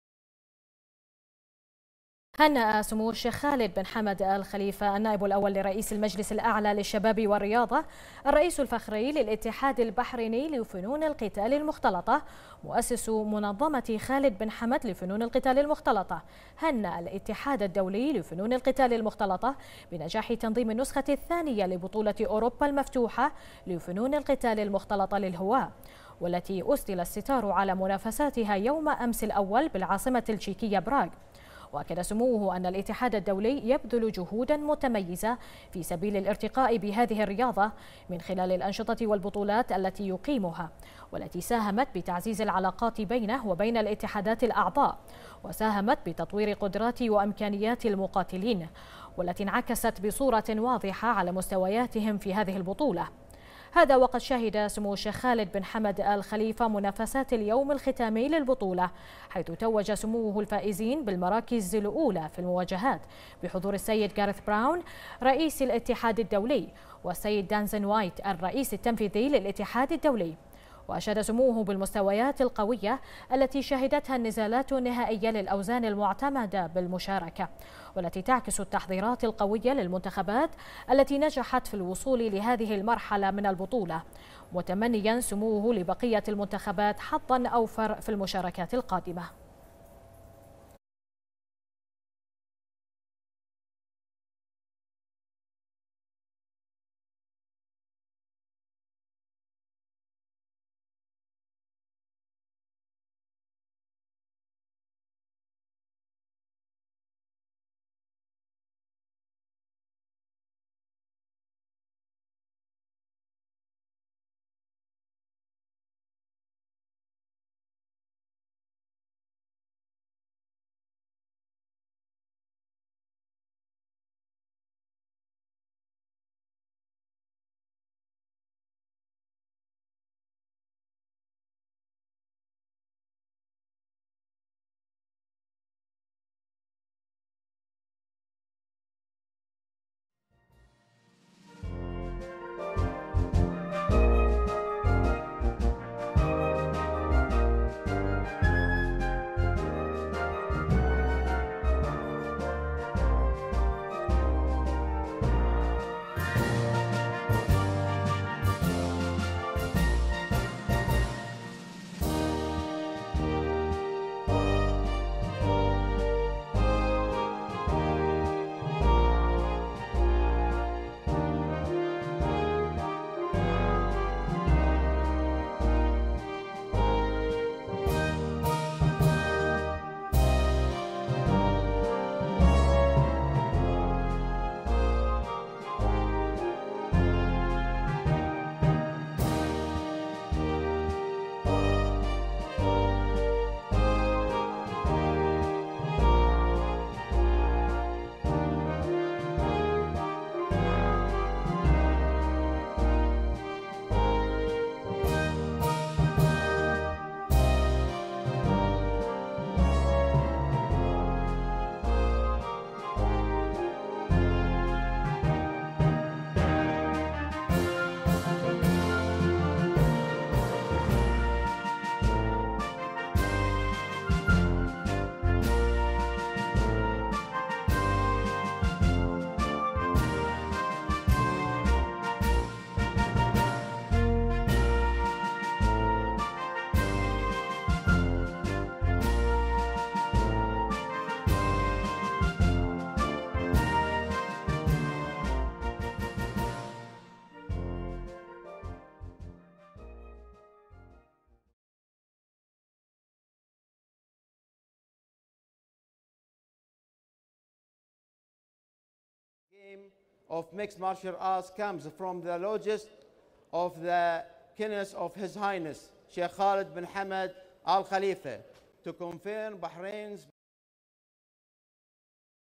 البحرين: سمو الشيخ خالد بن حمد يهنئ الإتحاد الدولي لفنون القتال المختلطة بنجاح تنظيم البطولة
براغ في 27 نوفمبر/ بنا / هنأ سمو الشيخ خالد بن حمد آل خليفة النائب الأول لرئيس المجلس الأعلى للشباب والرياضة الرئيس الفخري للاتحاد البحريني لفنون القتال المختلطة مؤسس منظمة خالد بن حمد لفنون القتال المختلطة، الاتحاد الدولي لفنون القتال المختلطة IMMAF، بنجاح تنظيم النسخة الثانية لبطولة أوروبا المفتوحة لفنون القتال المختلطة للهواة، والتي أسدل الستار على منافساتها بالعاصمة التشيكية "براغ".